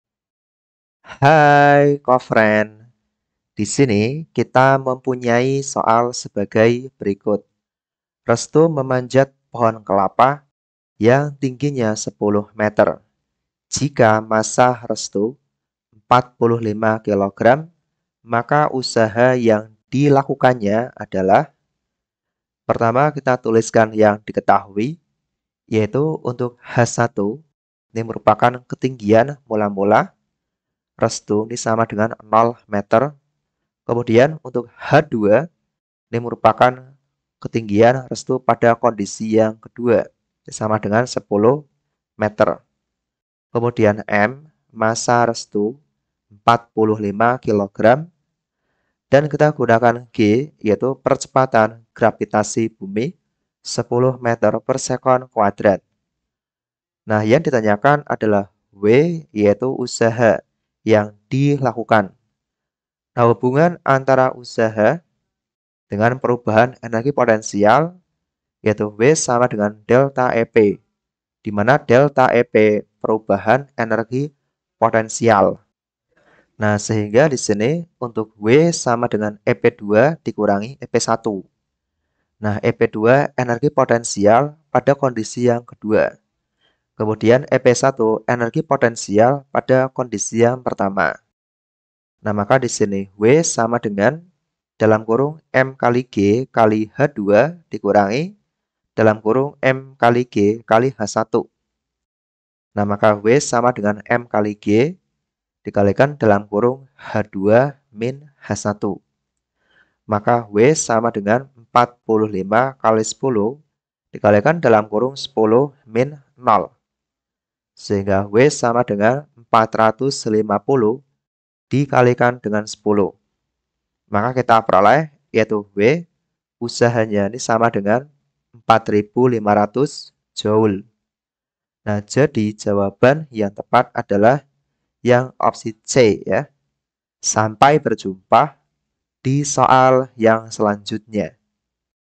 Restu memanjat pohon kelapa yang tingginya 10 m . Jika massa Restu 45 kg , maka usaha yang dila...
Restu memanjat pohon kelapa yang tingginya 10 m . Jika massa Restu 45 kg , maka usaha yang dilakukannya adalah .

Topik atau Materi: Usaha - Pesawat Sederhana - Mekanika - Fisika Kelas 8 SMP -

Sumber Soal: Student Input Apps Fisika Kelas 8 , N/A

00:00 : Bedah Soal
01:00 : Konsep, rumus dan pengertian Usaha
01:18 : Langkah penyelesaian soal
01:22 : Jawaban akhir
01:52 : Penutup